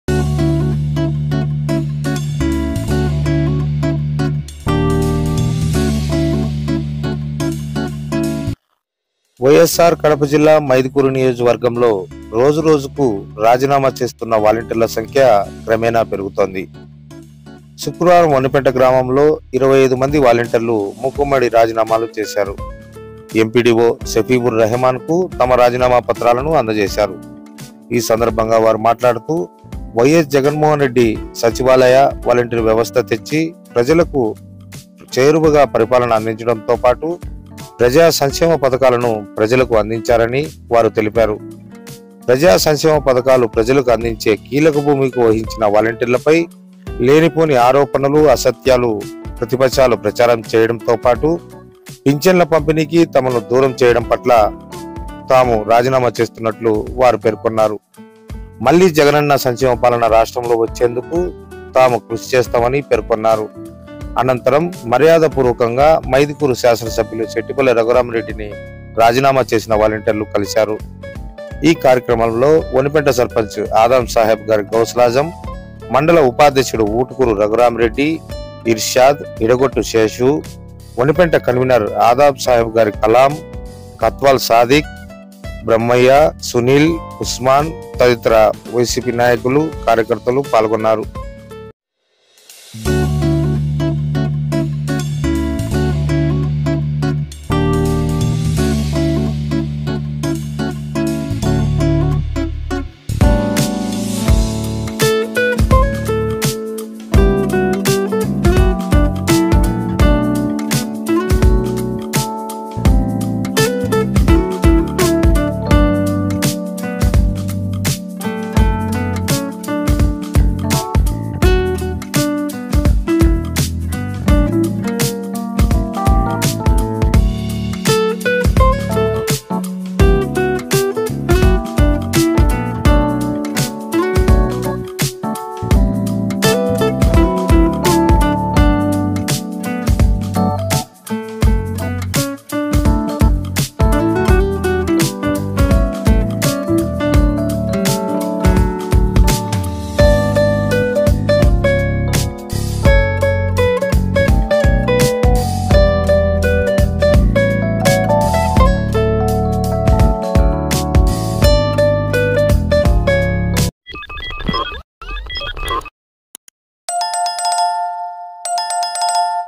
మైదుకూరు మండలం వనిపెంటలో వలంటీర్ల రాజీనామాలు || 25 volunteers resigned in Vanipenta village
మైదుకూరు నియోజకవర్గంలో కొనసాగుతున్న వలంటీర్ల రాజీనామా ల తీరు.

మైదుకూరు నియోజకవర్గం లో వలంటీర్ల రాజీనామా ల తీరు పెరుగుతున్నది. మైదుకూరు మండలం లోని వనిపెంట లో 25 మంది వాలంటీర్లు రాజీనామా చేశారు. @sahayanews1